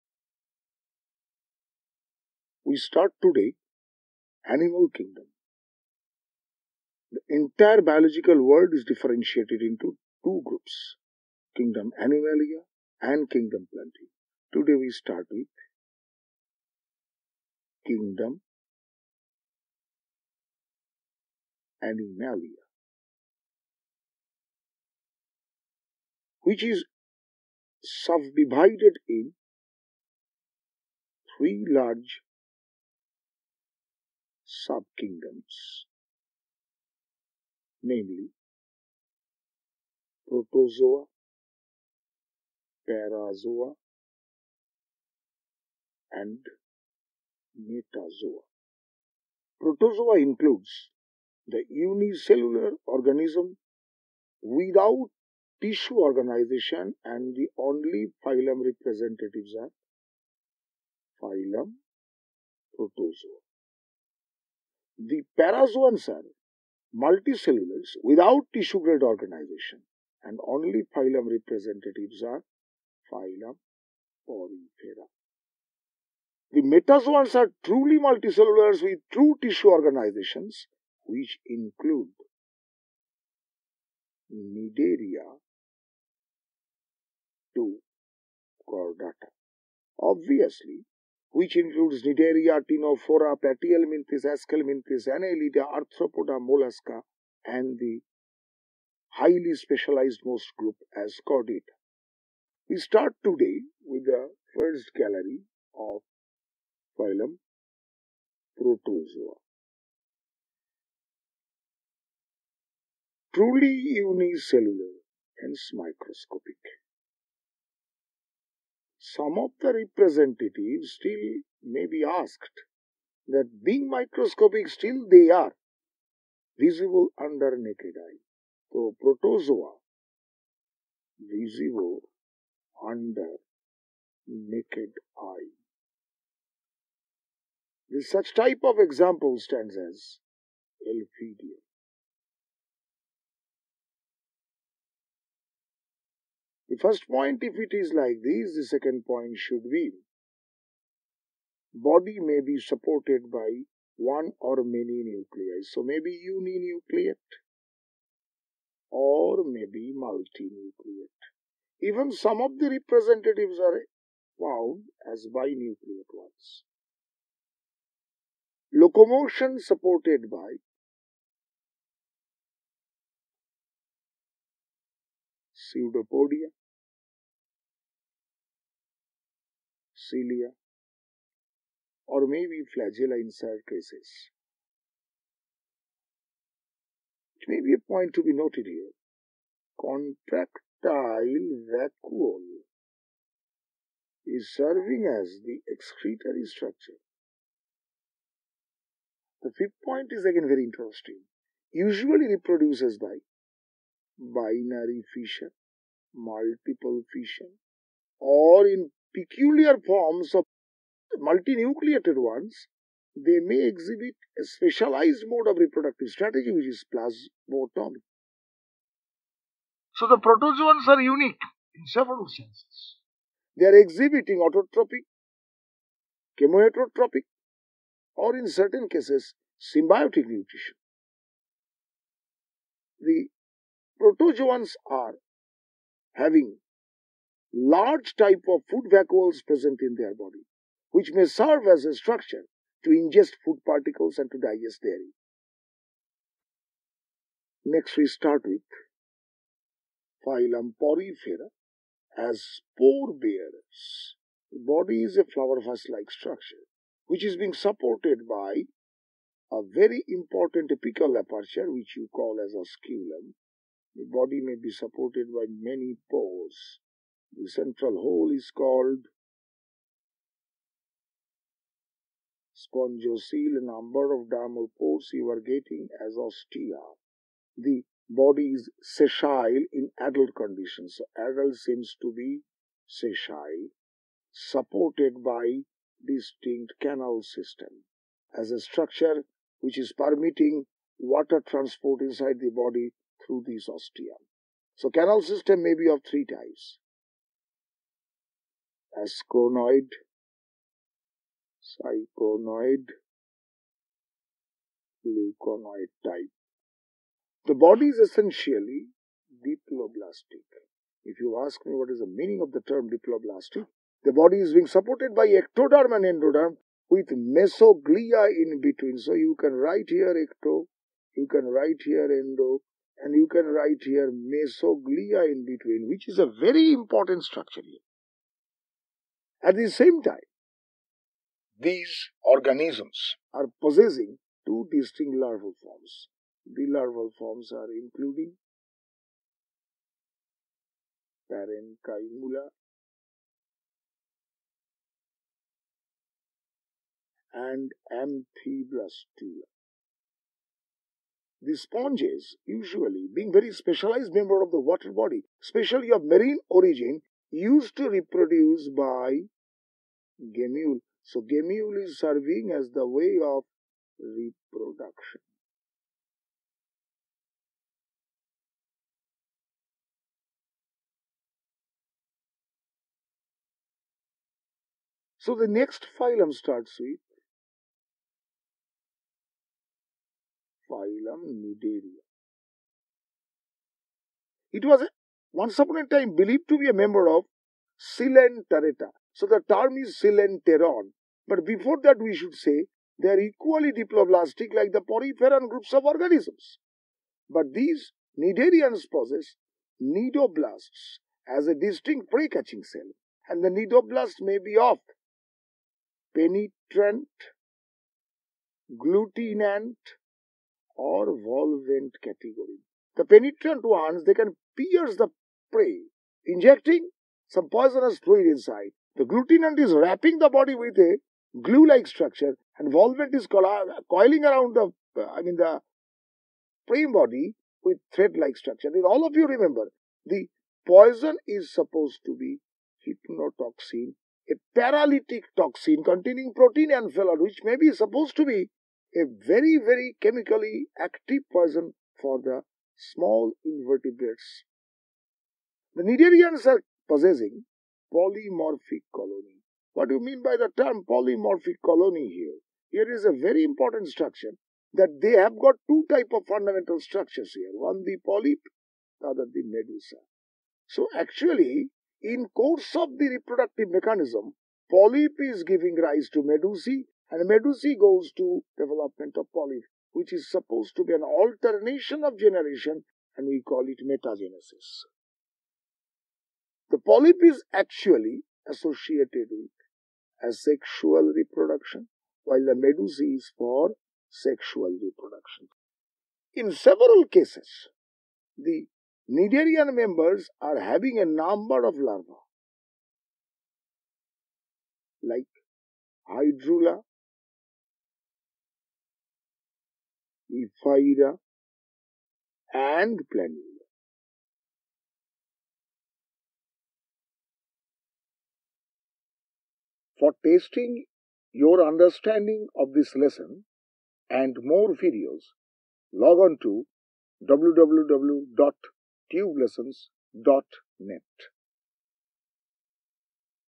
Animal kingdom | Protozoa | Parazoa | Metazoa
This video contains sub-division of animal kingdom Protozoa, Parazoa and Metazoa. Example of Protozoa, Parazoa and Metazoa. Detail about Protozoa.